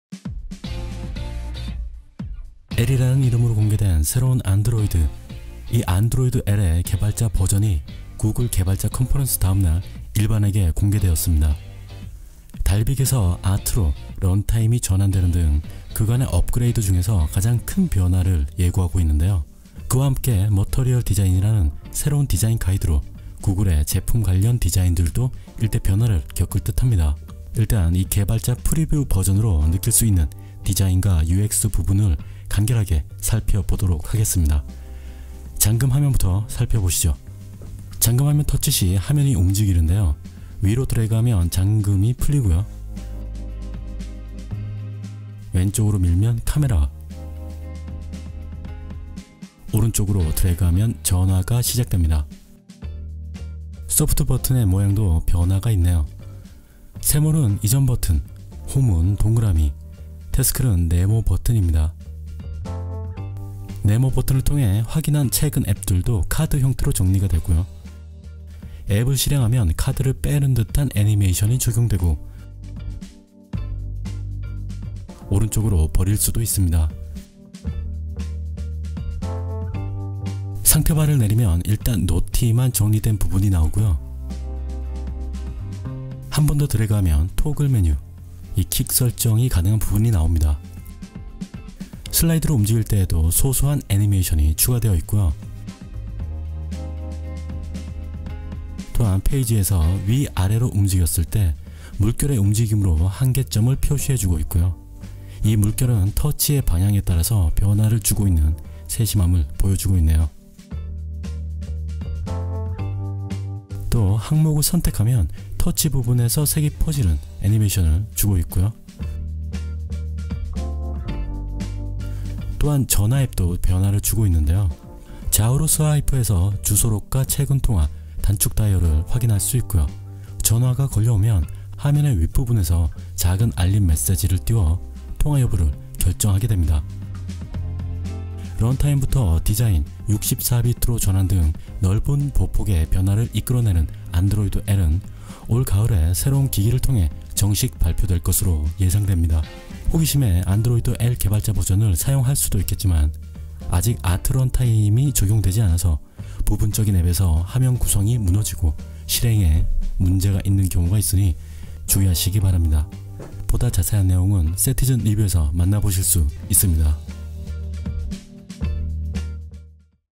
구글, '안드로이드 L'개발자 프리뷰 버전 배포
안드로이드 L의 개발자 버젼이 구글 개발자 컨퍼런스 다음날 일반에게 공개되었습니다.
달빅에서 아트로 런타임이 전환되는등 그간의 업그레이드중 가장 큰변화를 예고하고 있는데요.
그와함께 머터리얼 디자인이라는 새로운 디자인 가이드로 구글의 제품관련 디자인들도 일대 변화를 격을듯 합니다.
일단 이 개발자 프리뷰 버젼으로 느낄수있는 디자인과 UX부분을 간결하게 살펴보도록 하겠습니다.